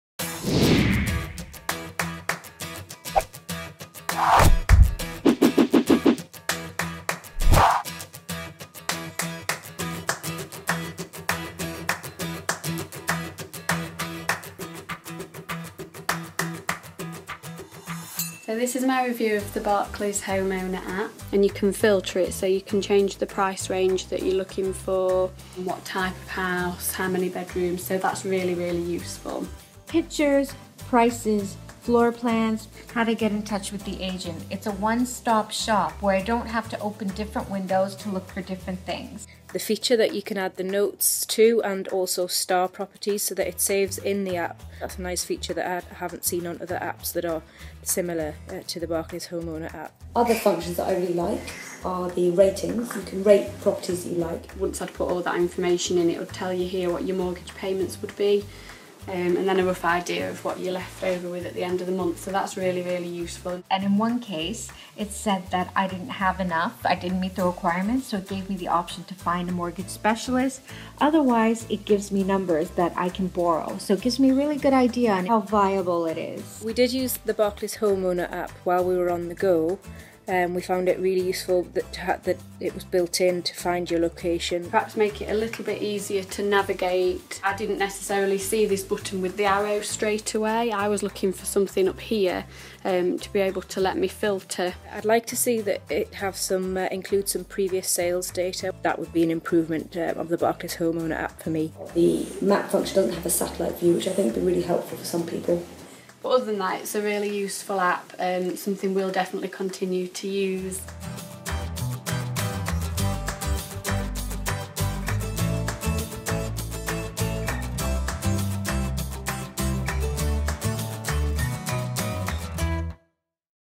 Barclays Homeowner App - Mumsnet Review
Barclays invited Mumsnetters to try out the Barclays Homeowner App and submit their feedback on camera. Here, they talk us through the key features - from the filtering options to the location-specific search - and share their thoughts on what could be improved.

If you have any thoughts on how Barclays could help families further, Barclays want to hear 'em!